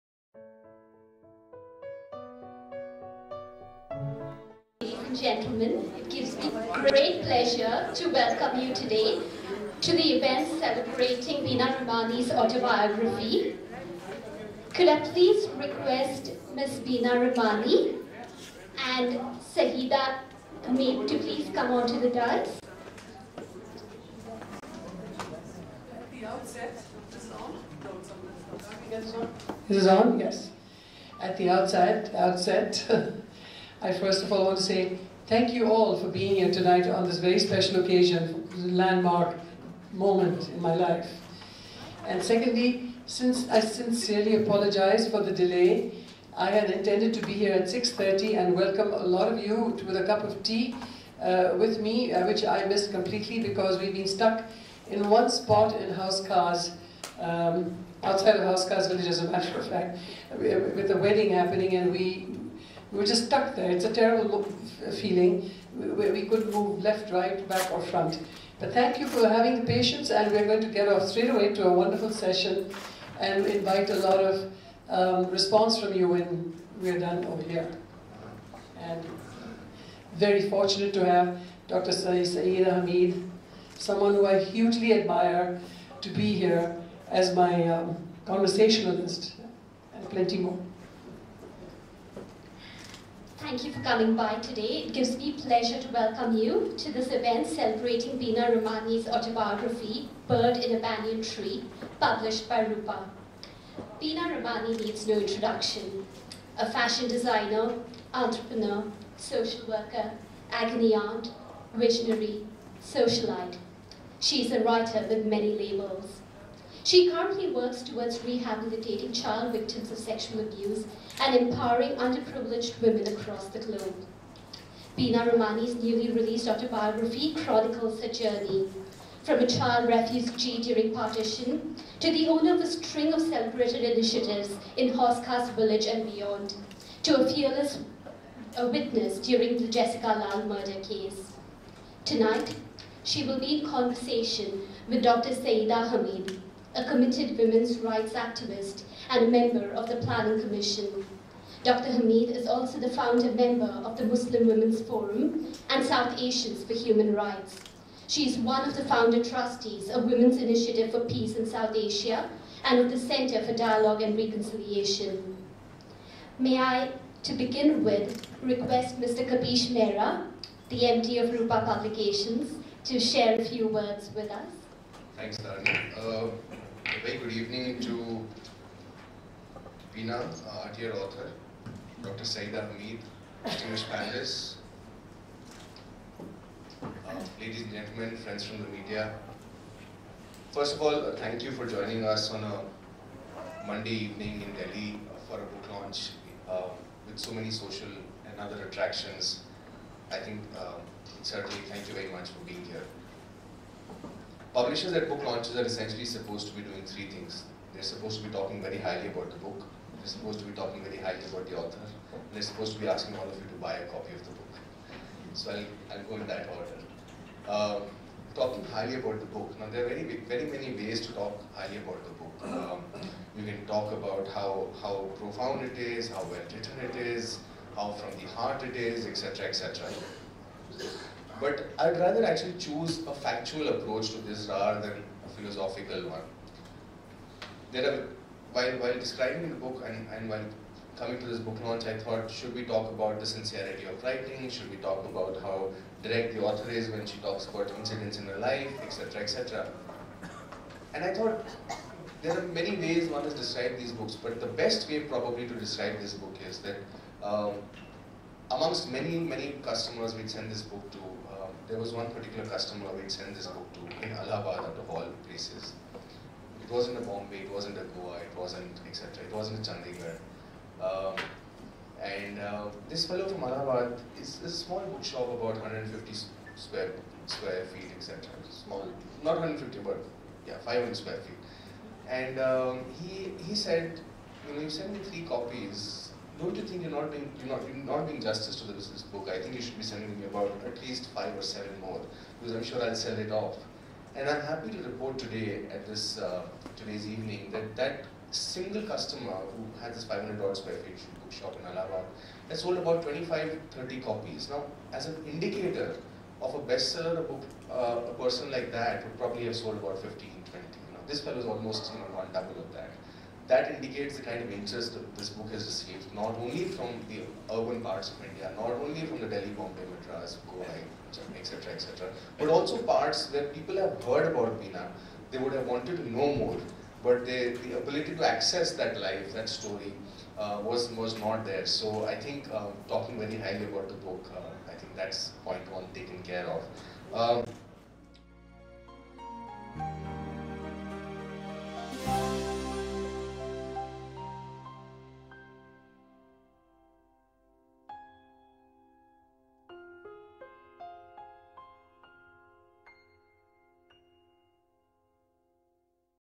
BOOK LAUNCH|Bird On A Banyan Tree by Bina Ramani 27th January 2014
BOOK LAUNCH|Bird On A Banyan Tree by Bina Ramani Fashion designer, entrepreneur, restaurateur, social worker, agony aunt, visionary, socialite—many labels have been applied to Bina Ramani. But what is her real story? From arriving as a refugee child in India during Partition to having Binatone, a successful electronics brand named after her; from a nine-year-old sitting on her favourite black rock, watching the waves and weaving dreams in Mumbai, to a passionate relationship with screen idol Shammi Kapoor; from a loveless marriage and a bitter divorce, to opening her own stores and changing the face of Delhi's Hauz Khas Village; from searching for her daughters across continents, to being taken in police custody and sent to Tihar Jail for being a stubbornly honest witness; from being vilified by the media to being lauded by the country for her heroic role in the Jessica Lal murder case—in this inspiring, no-holds-barred memoir, Bina tells it all. Collab: Rainlight, Rupa Publications